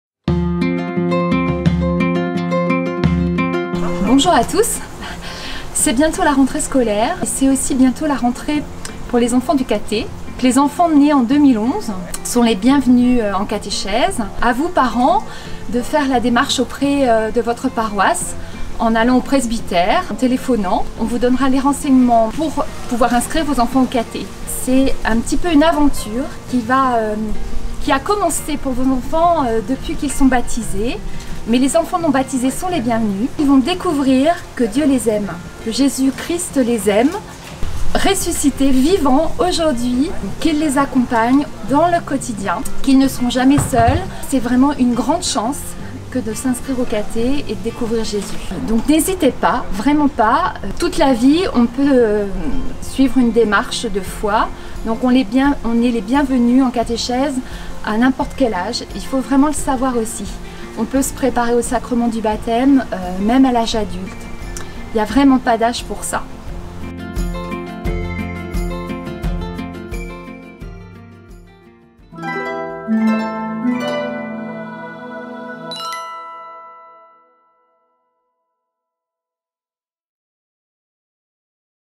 C'est la rentrée du Caté !
📆 Votre RDV vidéo avec "La rentrée du Caté” !

Aujourd’hui : Bientôt la rentrée des classes ! Interessé pour rejoindre le catéchisme ?

➡ N’oubliez pas, chaque semaine un nouveau thème (actualité, enseignement, événements, etc)